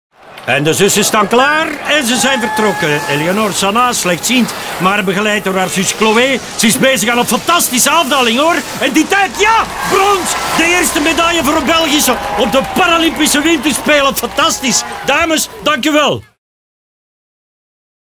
Skiër Eléonor Sana
In 2018 was Eléonor Sana de eerste Belgische vrouw die een medaille behaalde op de Paralympische Winterspelen. Dit historisch moment geeft ons nog steeds kippenvel! Dat is waarom wij al 10 jaar lang supporter zijn van het Paralympic Team Belgium. En we kijken reikhalzend uit naar de volgende competities, onze stembanden zijn gesmeerd 😉.